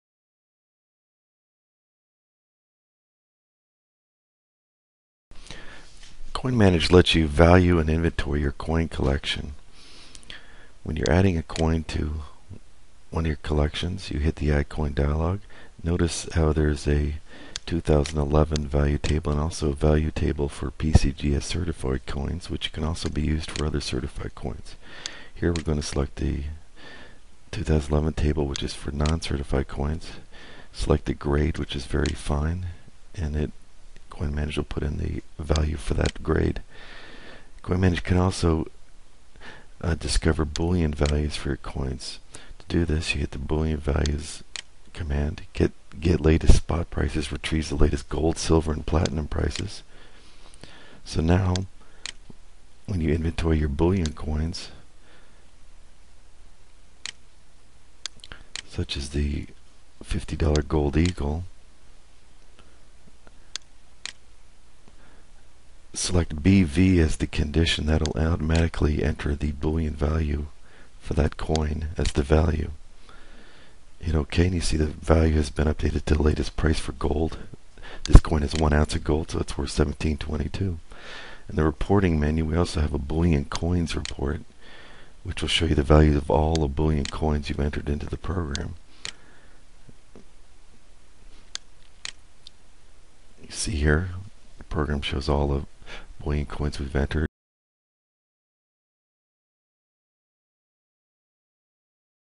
CoinManage Coin Software - How to Value your Coins
Tutorial on how to use CoinManage coin collecting software to value your coin collection.